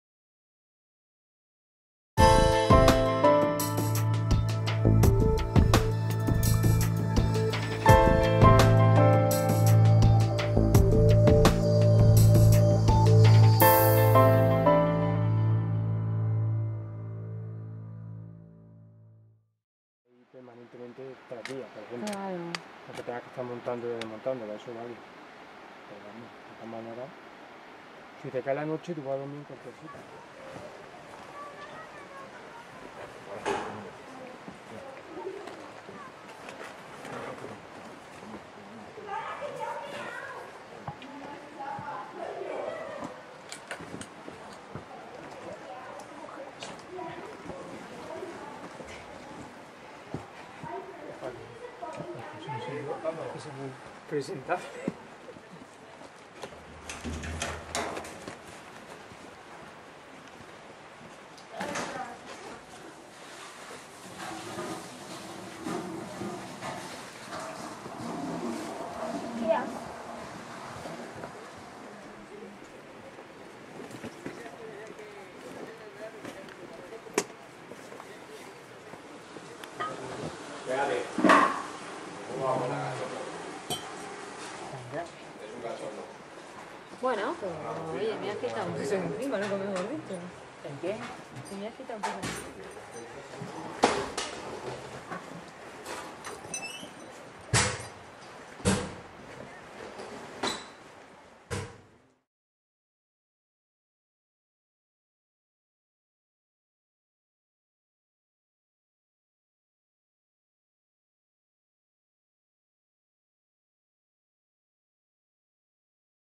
Acentor Alpino (Sierra de Gredos)
Avistamiento de un Acentor Alpino (Prunella collaris) en las proximidades del Refugio Elola de la Laguna Grande de Gredos (Avila) a 1.970 m. de altitud. Grabación realizada por Miguel Ángel López Varona el 25 de octubre de 2009 para el Blog "Cuaderno de Campo del Treparricos", después de la ascensión al Pico Almanzor. Más información en: htpp://